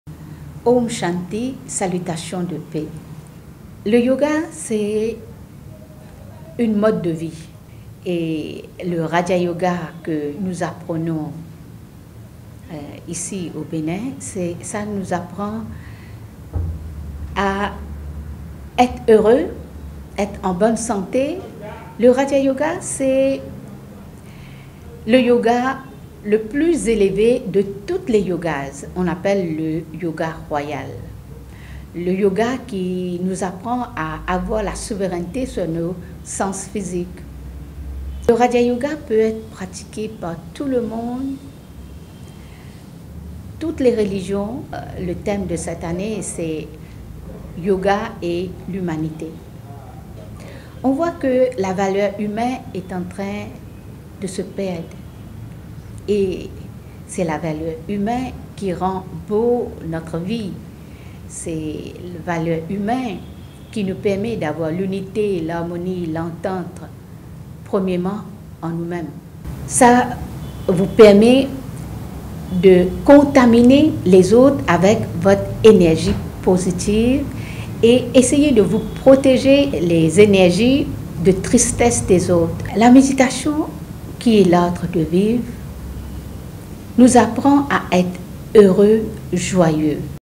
Yoga : une pratique qui gagne du terrain
21 juin, c’est la journée internationale du yoga. Le yoga, un mode de vie de plus en plus adopté par de nombreuses personnes. Pour certains c’est une pratique spirituelle, pour d’autres une forme de sport.  Dans cet extrait que nous vous proposons, la Soeu Vinadevi du centre de yoga nous dit ce qu’elle entend par yoga.